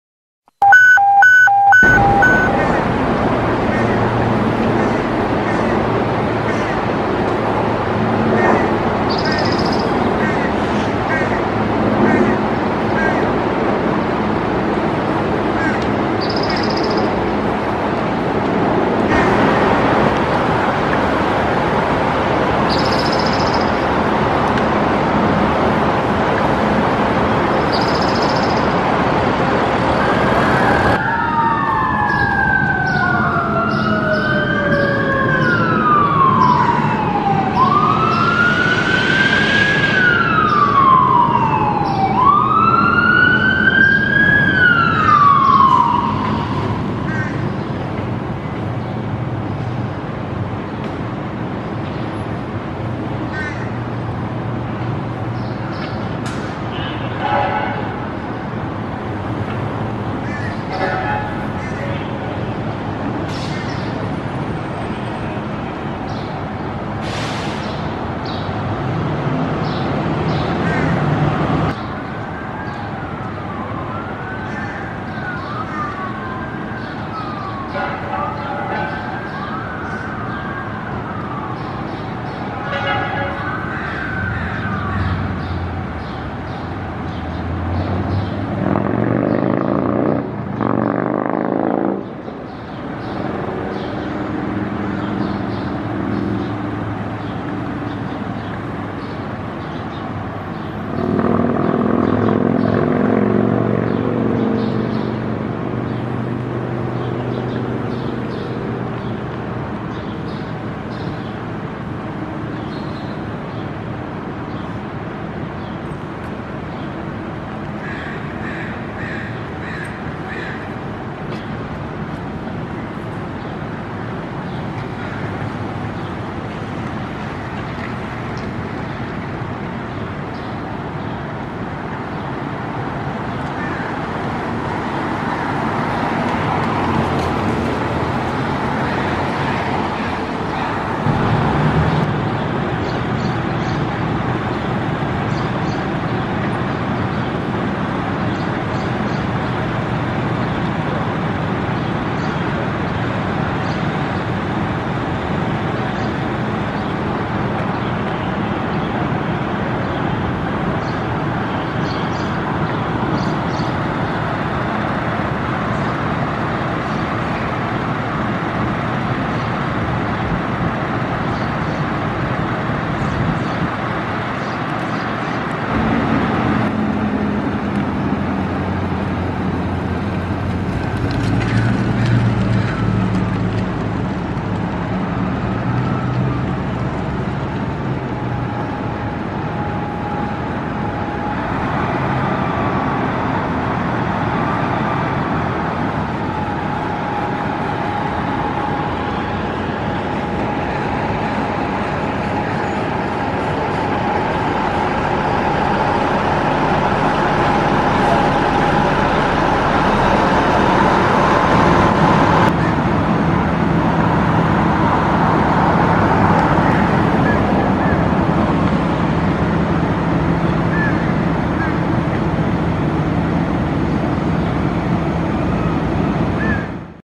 West Van Minor Brush Fire Scene Footage
West Vancouver Fire & Rescue Engine 3, Command 1 (Duty Chief) & Old Tower 1 arriving for a brush fire next to a high rise. Special thank you to my friend Anthony for sending this video.